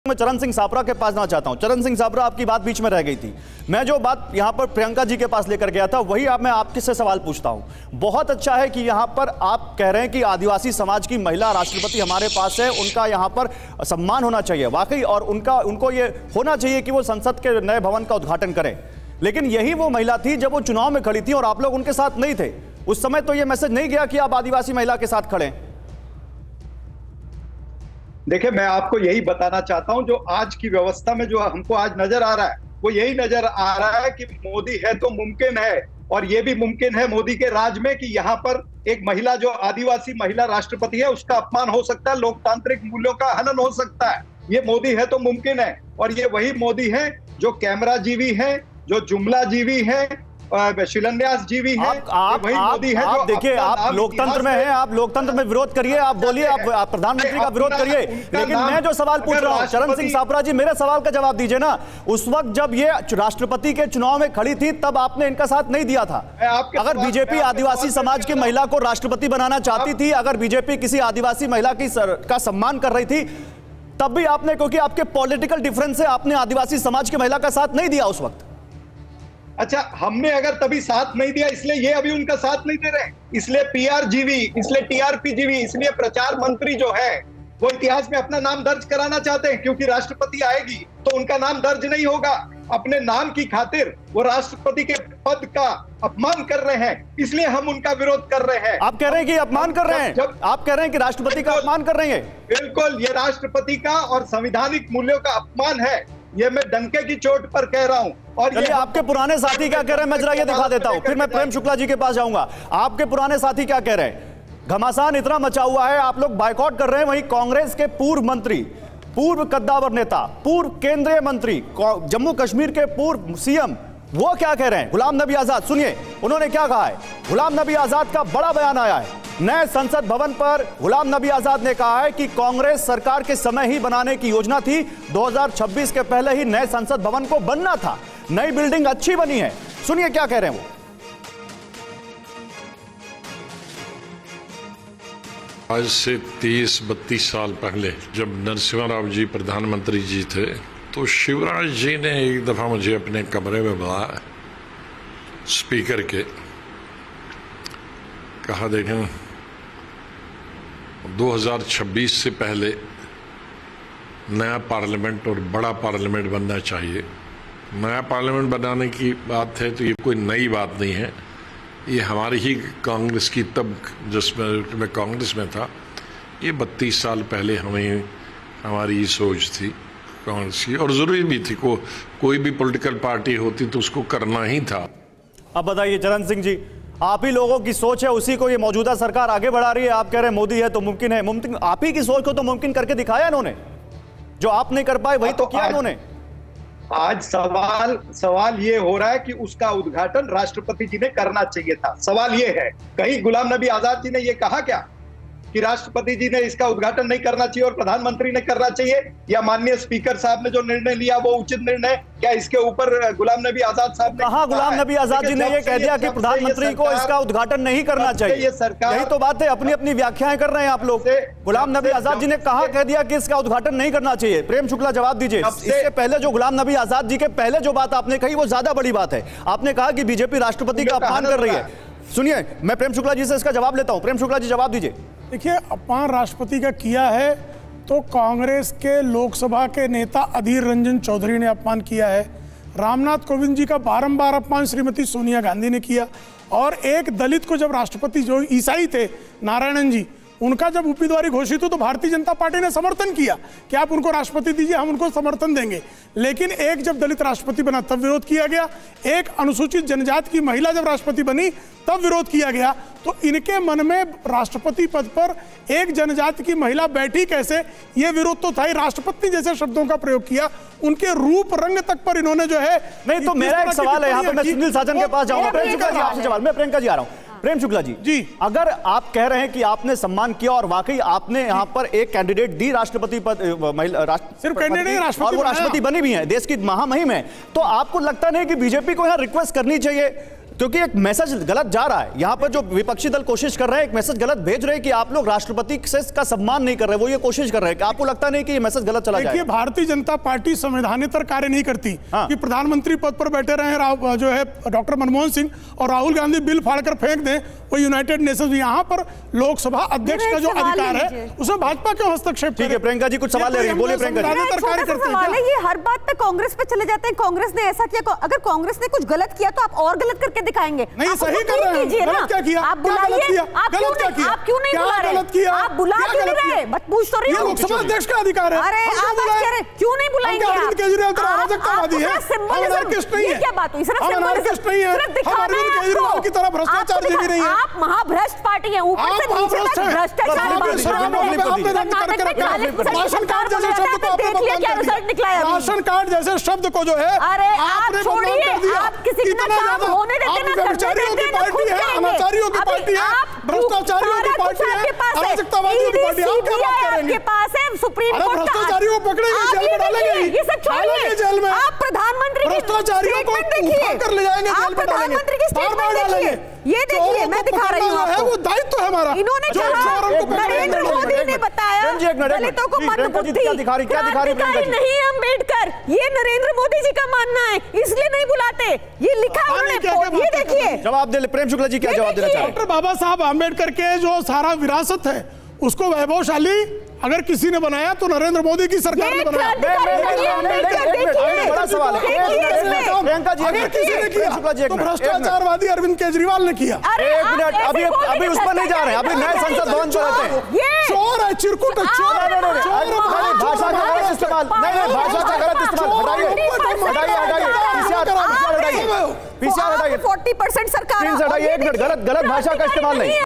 New Parliament Buildingपर गुलाम नबी आजाद का बड़ा बयान 'कांग्रेस सरकार के समय ही बनाने की योजना थी'
नए संसद भवन के उद्घाटन को लेकर घमासान मचा हुआ है.कांग्रेस ने समारोह का बायकॉट किया है..वहीं कांग्रेस के पूर्व मंत्री और कद्दावर नेता रहे गुलाम नबी आजाद ने नए संसद भवन की जरूरत बताई है.